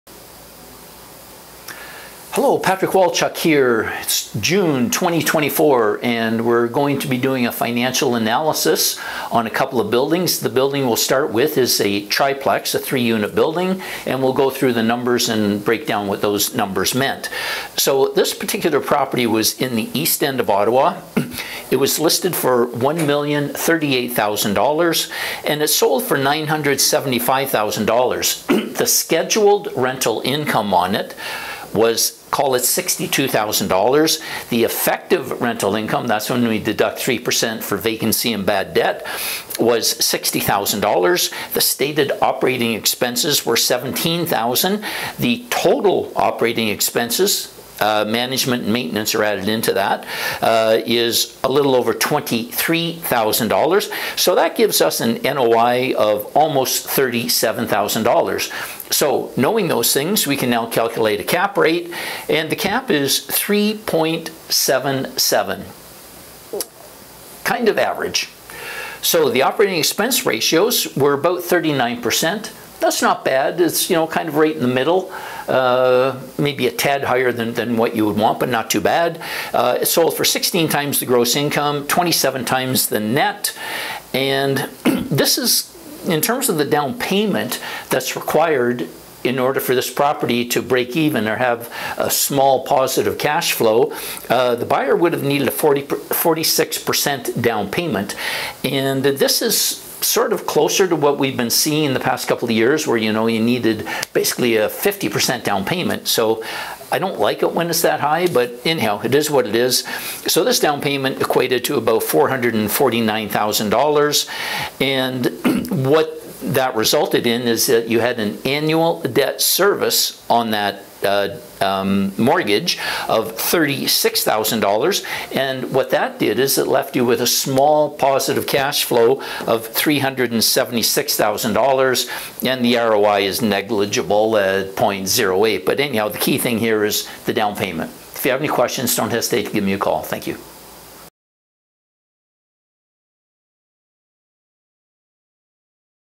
Financial Analysis, East Ottawa Triplex Sold June 2024
Financial information from the sale of this East Ottawa Triplex that SOLD in June, 2024.  This property is located in east Ottawa, with one 1-bedroom unit and two 2-bedroom units.

The property was listed for $1,038,000 and sold for $975,000.

Scheduled Rental Income was $61,920
Effective Rental Income was $60,062
Operating expenses of $17,332
Total Operating Expenses, with 10% for maintenance/management $23,338
Net operating income of $36,724
Cap Rate was 3.77%
Operating Expense Ratio was 38.9%
Sold for 16 times the Gross Income
Sold for 27 times the Net Income
With a 46% down payment of $448,500, this leaves a mortgage of $526,500. At a 4.89% rate, amortized over 25 years, the monthly mortgage payment would be $3,029, for an Annual Debt Service of $36,348 (this is the annual mortgage payment).

The final cash flow on this building was $376 and the ROI (return on investment) was 0.08%.